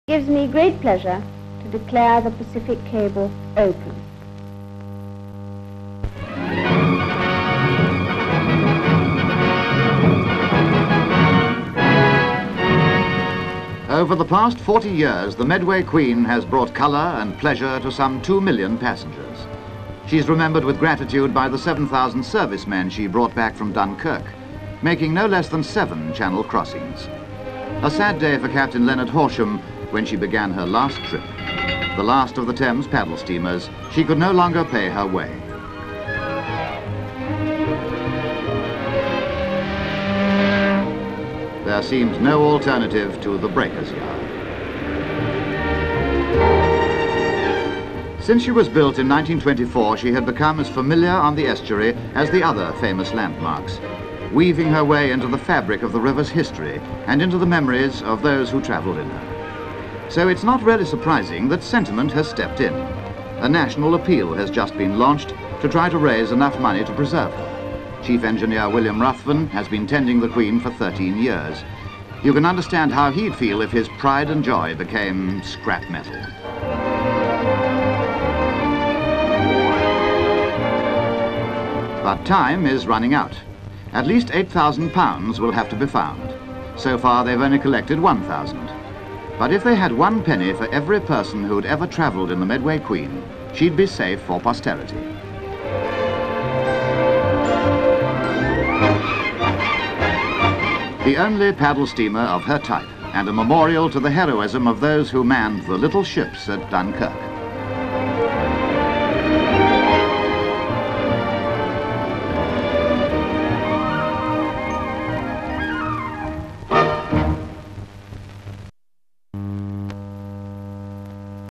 LAST OF THE PADDLE STEAMERS (IN COLOUR)
(5 Dec 1963) A National Appeal has just been launched to try to raise enough money to preserve "Medway Queen", the paddle steamer, that has brought colour and pleasure to some 2 million passengers over the past 40 years. She's remembered with gratitude by the 7,000 servicemen she brought back from Dunkirk, making no less than 7 channel crossings. It was a sad day for Captain Leonard Horsham, when she began her last trip this Summer. The last of the Thames paddle steamers, she could no longer pay her way, and there seemed no alternative than the breakers' yard. Will the "Medway Queen" have to go that sad, last way?